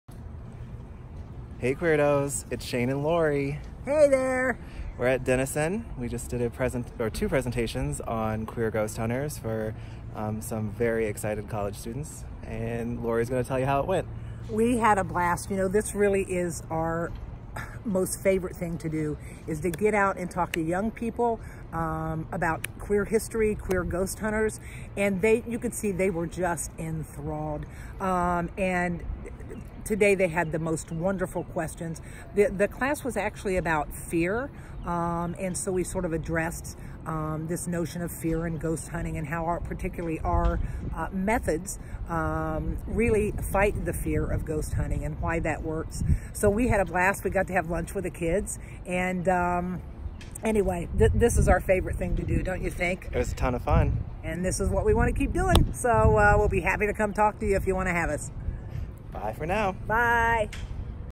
The Queer Ghost Hunters speak to college students
Queer Ghost Hunters Lori Gum and Shane McClelland speak at Denison University!